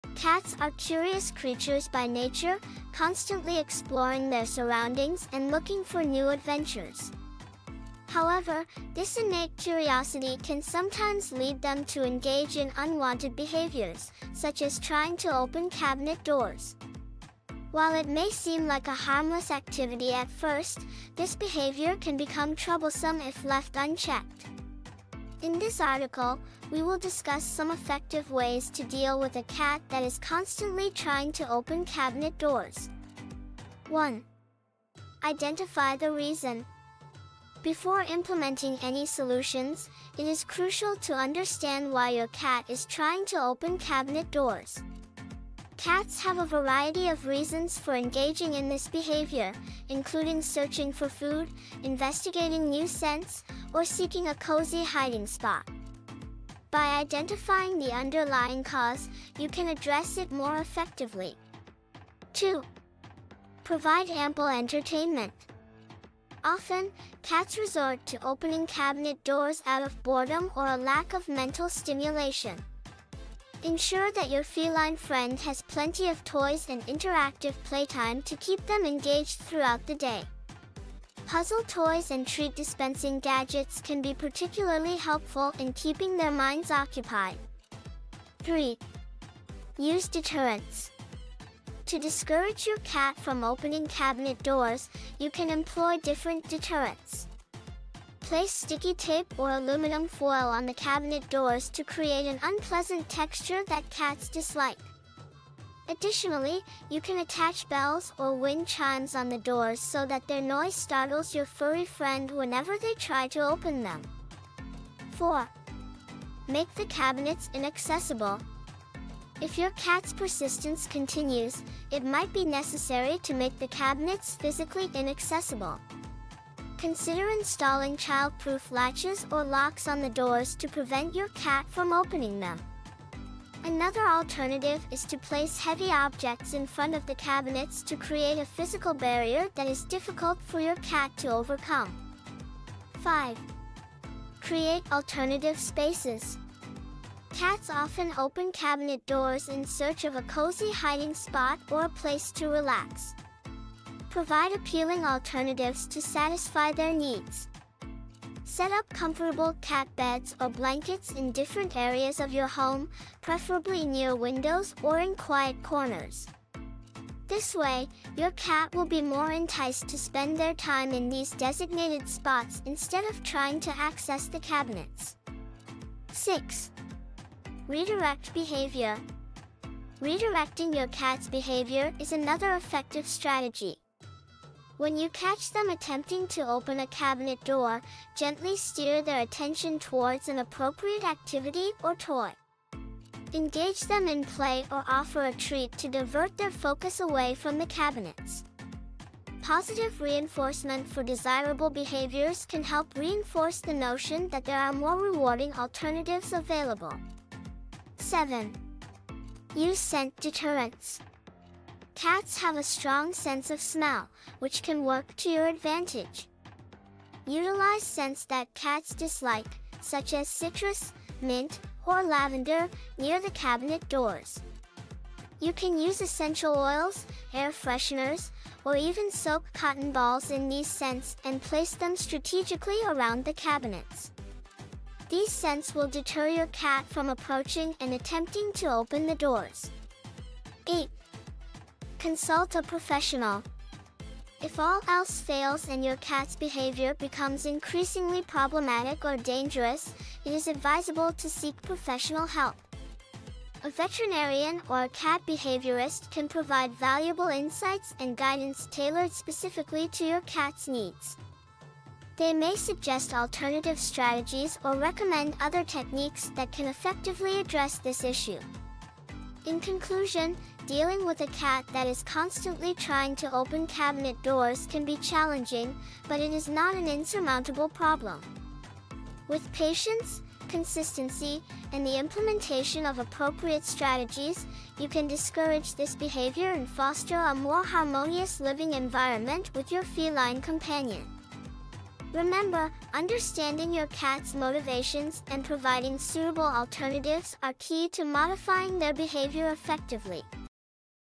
How to Stop Your Cat from Opening Cabinet Doors!
This article explores the curious nature of cats and how it can sometimes lead to unwanted behaviors, such as trying to open cabinet doors. It offers tips on how to prevent this activity and provides solutions to keep cats safe. Keywords: cats, curiosity, unwanted behaviors, cabinet doors, prevention, solutions, safety.